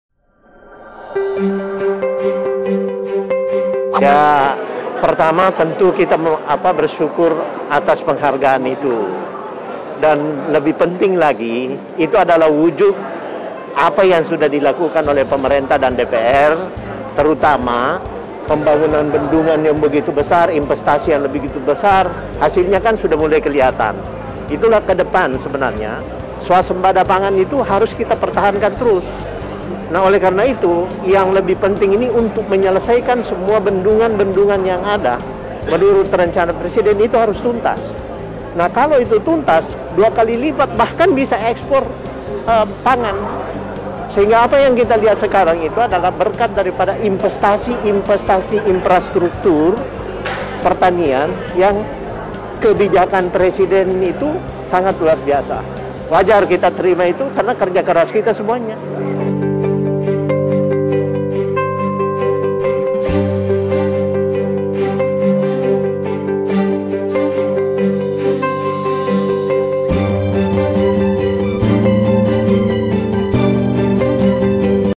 PEMBANGUNAN INFRASTRUKTUR PERTANIAN HARUS TERUS DILANJUTKAN UNTUK WUJUDKAN SWASEMBADA PANGAN
Anggota Komisi V DPR RI, Hamka Baco Kady mendorong pemerintah untuk terus melanjutkan pembangunan infrastruktur di bidang pertanian. Menurutnya infrastruktur pertanian adalah modal berharga untuk mencapai swasembada pangan.